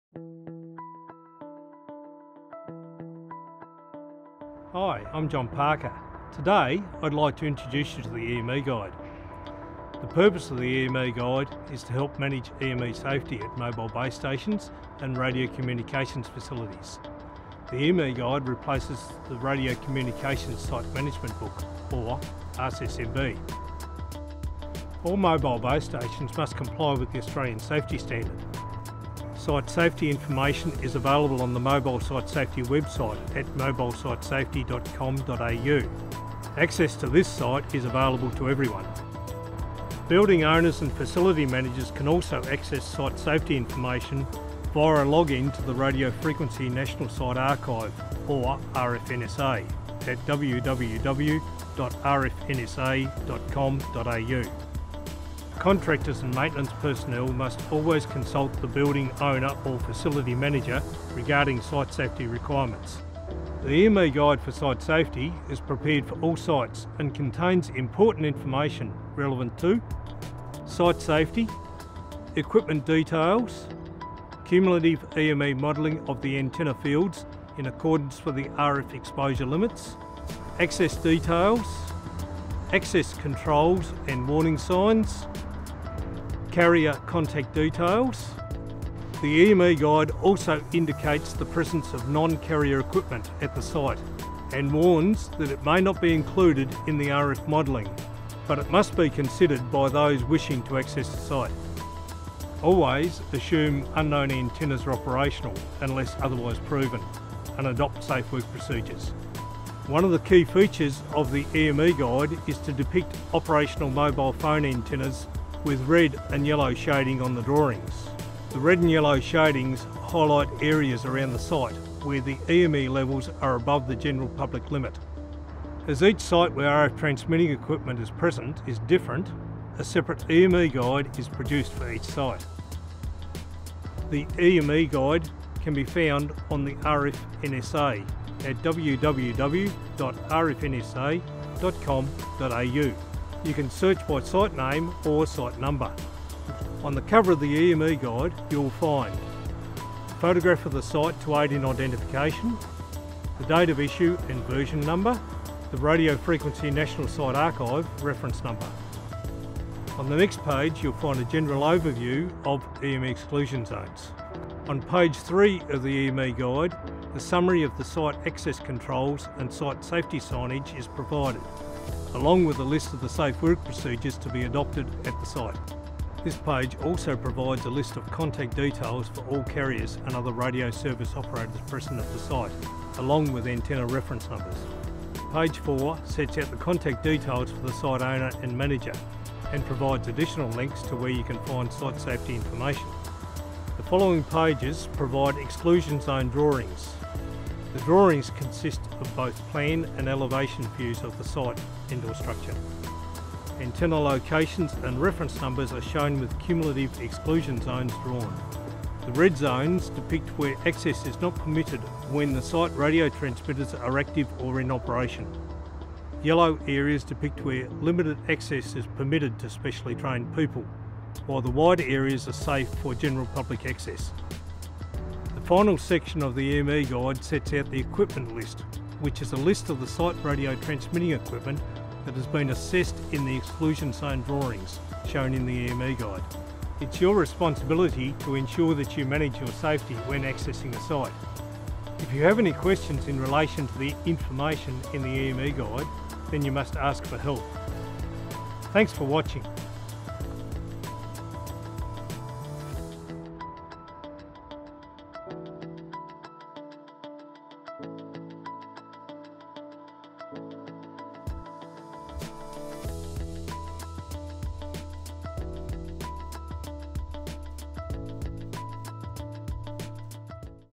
Introducing the EME Guide for site safety at telecommunication facilities
John Parker introduces you to the EME Guide. The EME Guide is  used to help manage electromagnetic energy safety at mobile base stations and radio communications facilities.  The EME Guide replaces the Radio Communications Site Management Book or RCSMB.